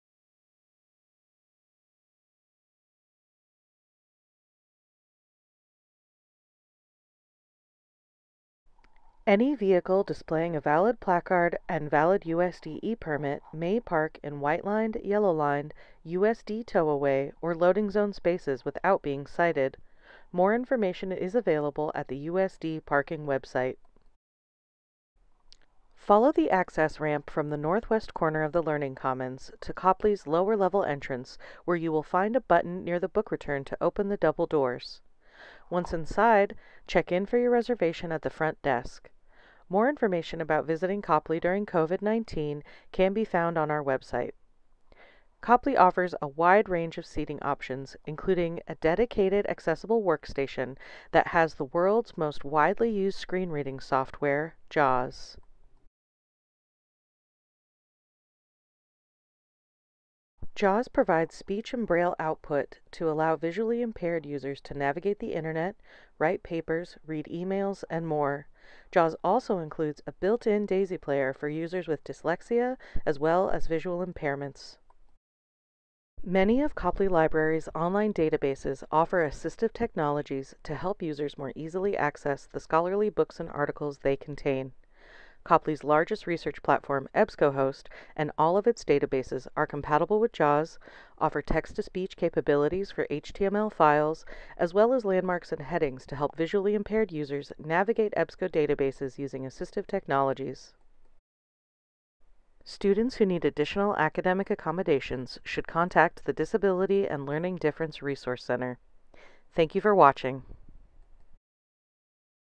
Accessibility at Copley Library (Spring 2021)
This video provides an overview of accessibility at Copley Library from parking and building access, to our most popular software and database.